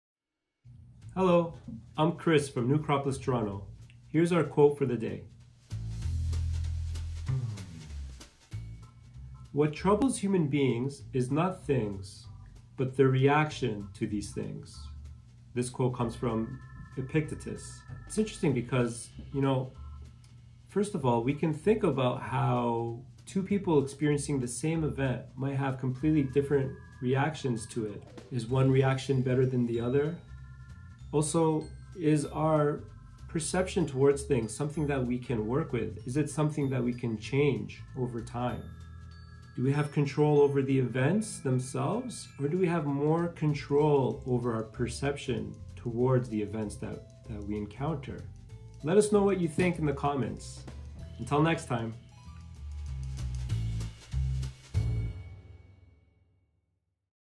Quote of the day - What's troubling us?
"What's trouble human beings is not things, but their reaction to these things" - Epictetus.